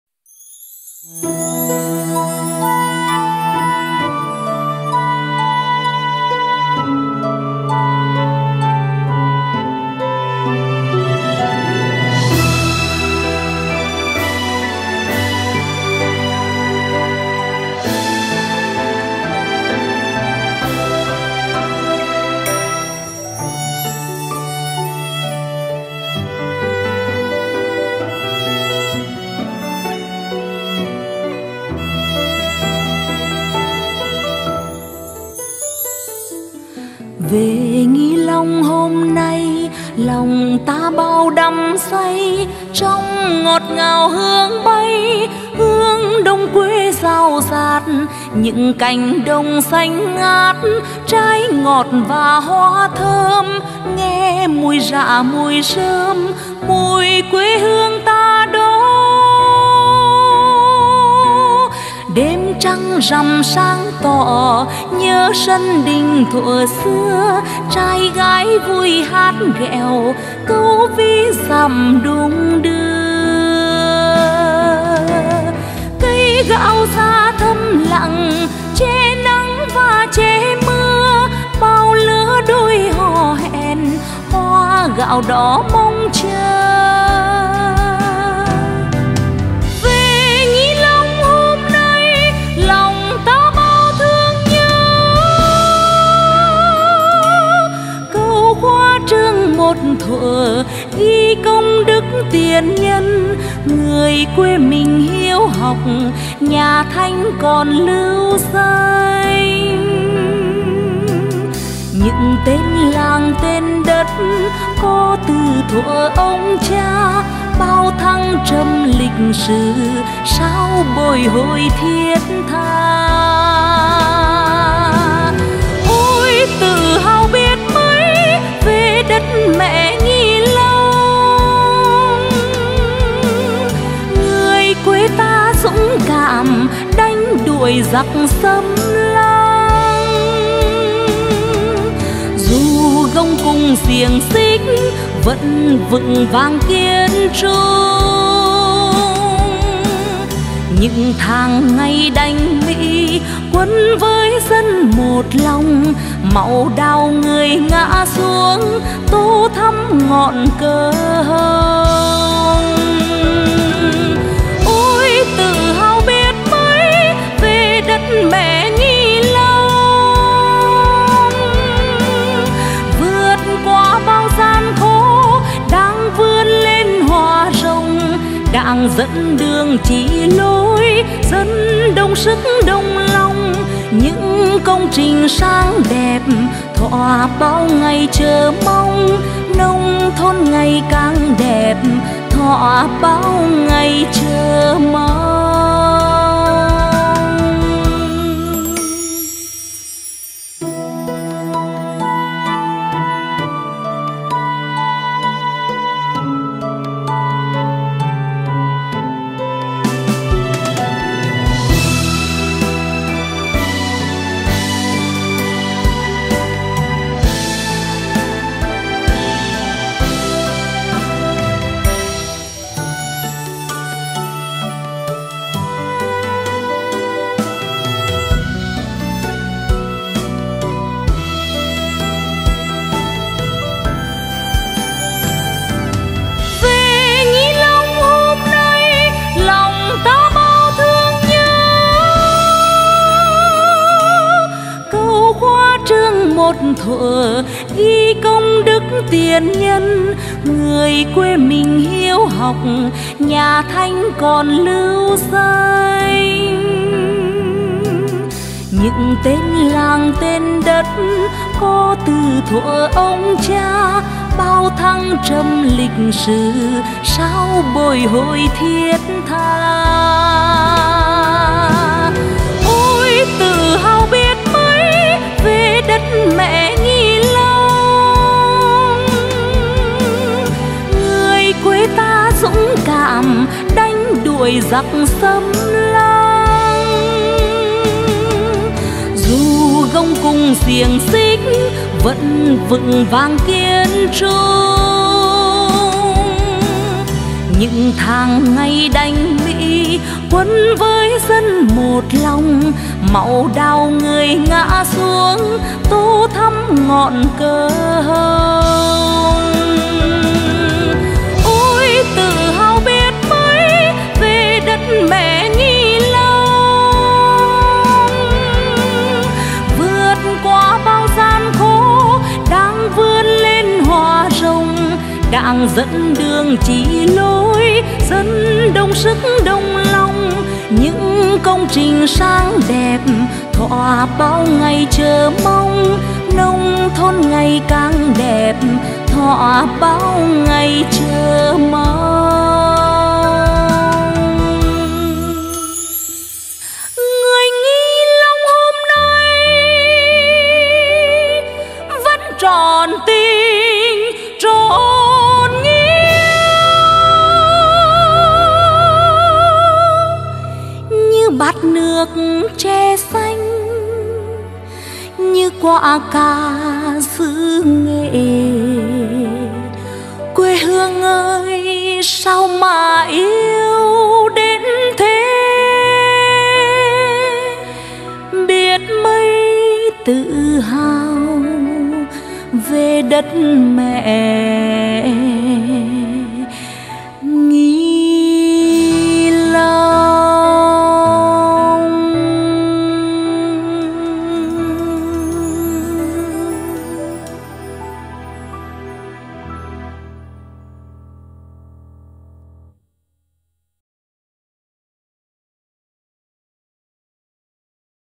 TỰ HÀO ĐẤT MẸ NGHI LONG
Chúc mừng xã Nghi Long là xã đầu tiên của huyện Nghi Lộc được UBND tỉnh Nghệ An công nhận đạt chuẩn nông thôn mới nâng cao năm 2022.